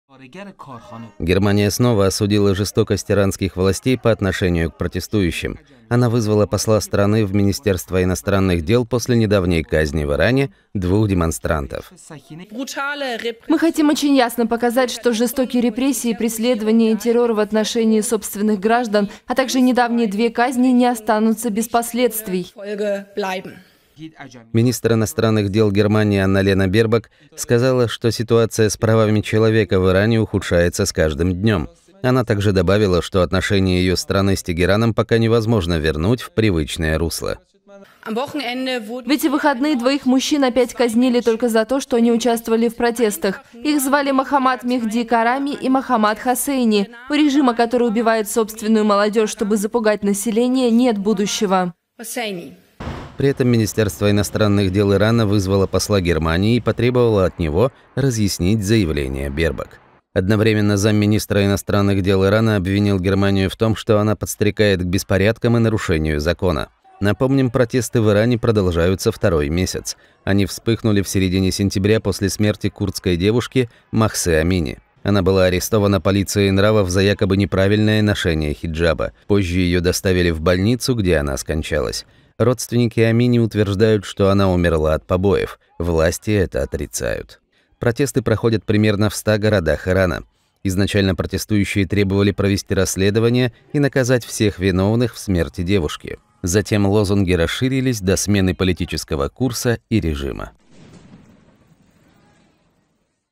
Германия вызвала посла Ирана после очередной казни протестующих
МИД Германии вызвал посла Ирана для дачи объяснений после казни в стране ещё двоих протестующих. При этом внешнеполитическое ведомство Ирана вызвало немецкого посла, чтобы он объяснил заявление главы МИД Германии Анналены Бербок.